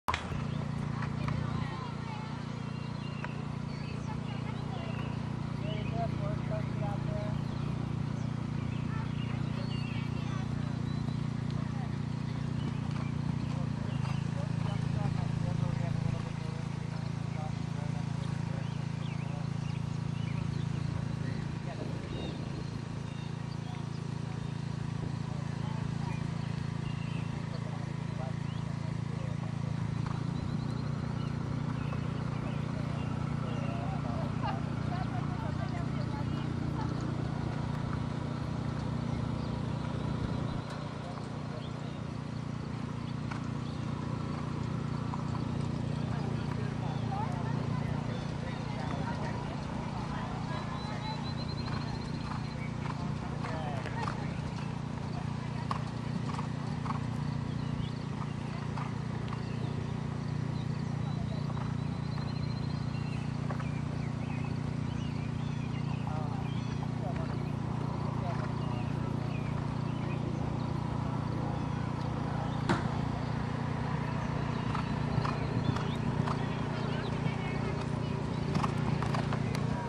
Toy Loudoun Benefit
Toy Town at his second a show in June 2006. Ribboned in classes of nearly 40.